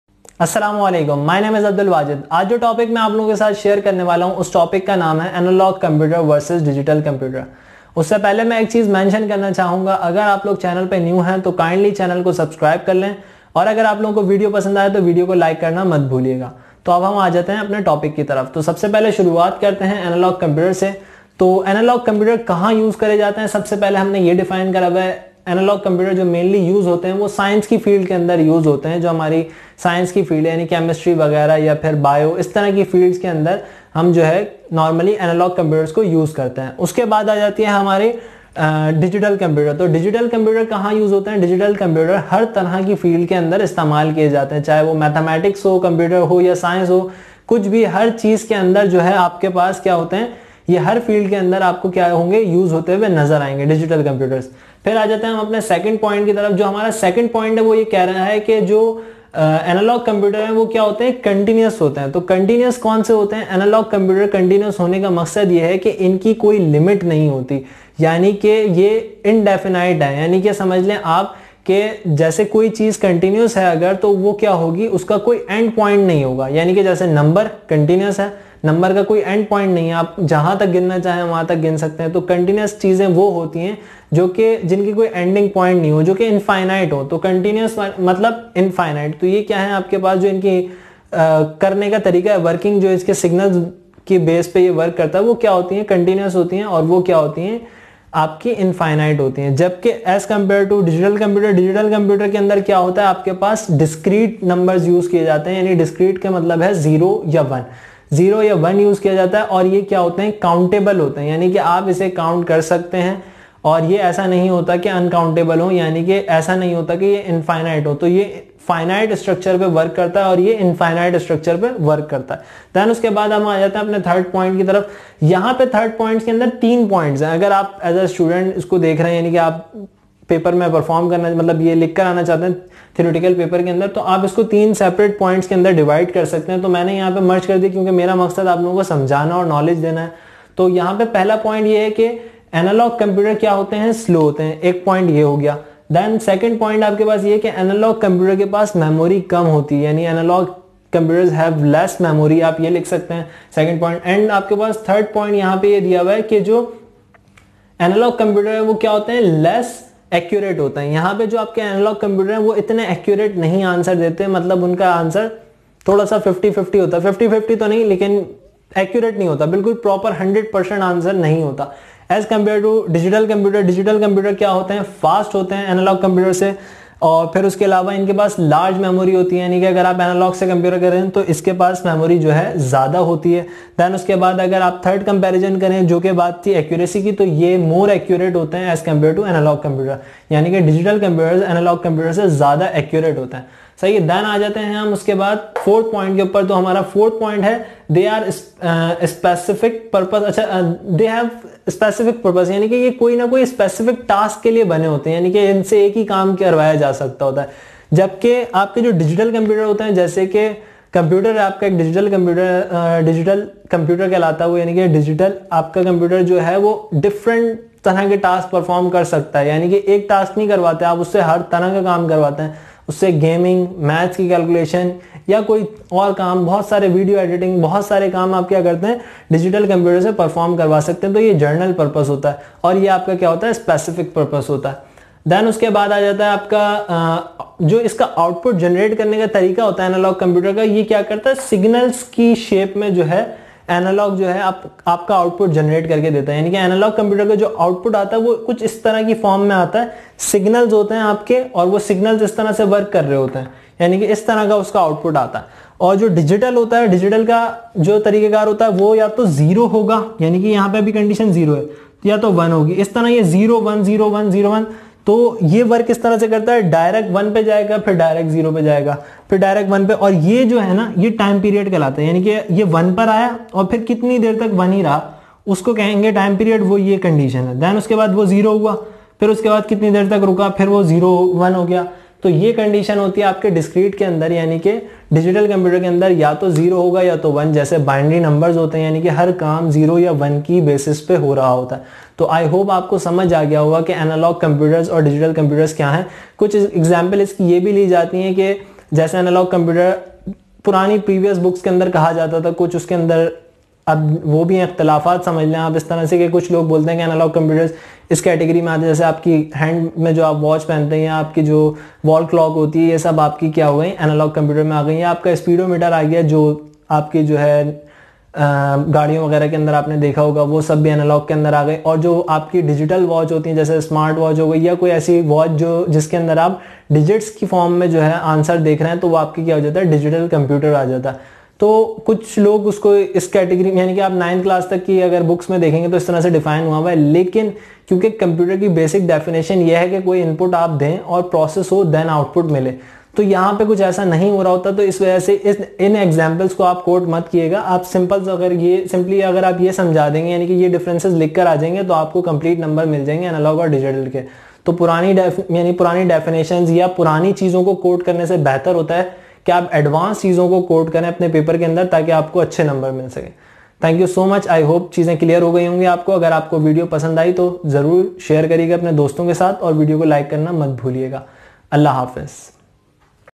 Difference Between Analog Computer and Digital Computer By Abdul Wajid Urdu/Hindi
This video provides the knowledge of Analog and Digital computer. the difference between Analog computer and Digital computer is provided with the points . so easily learn the difference between Analog and Digital computer